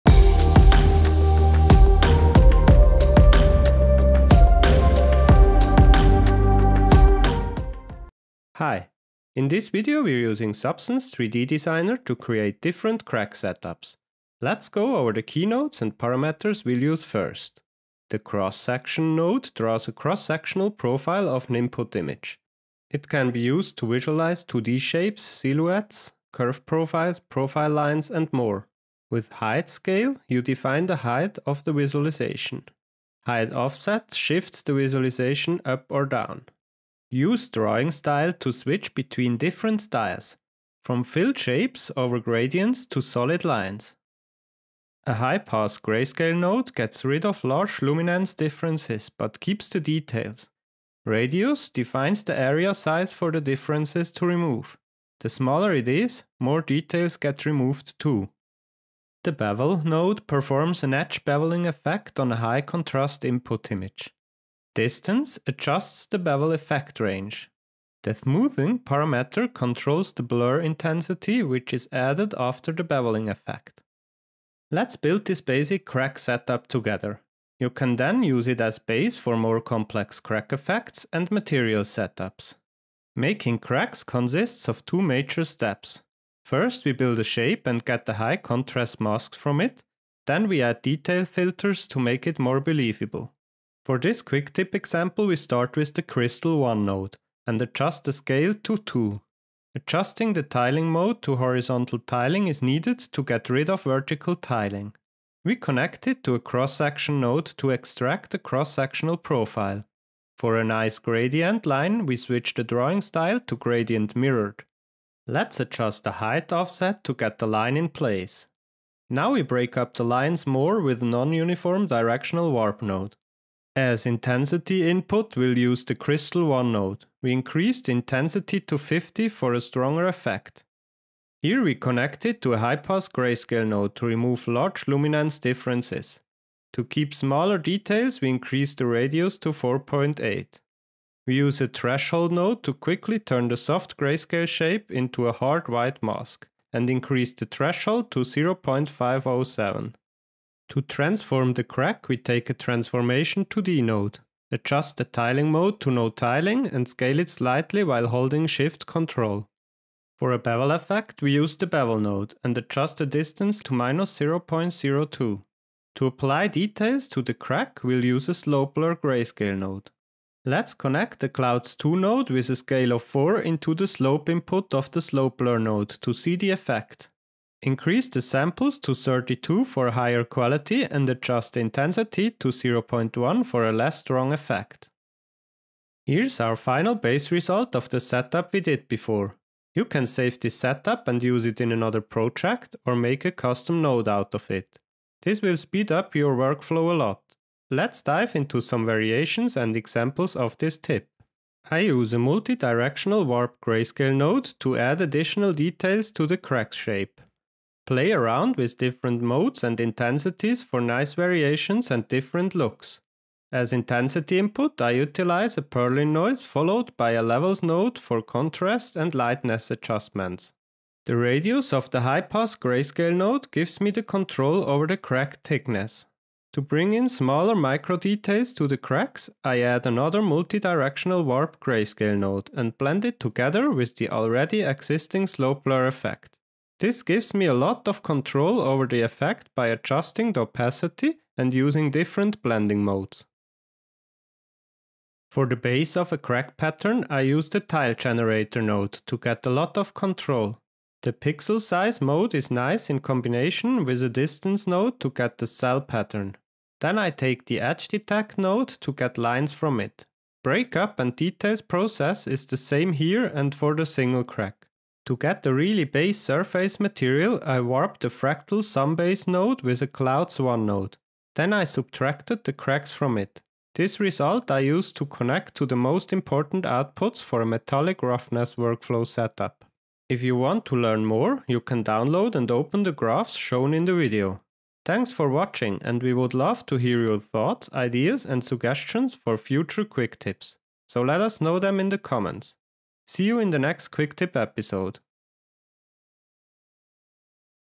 Cracks | Designer Quick Tip #8 | Adobe Substance 3D
In this video we're using Substance 3D Designer to create different cracks setups.

Voice and video by Martin Schmitter.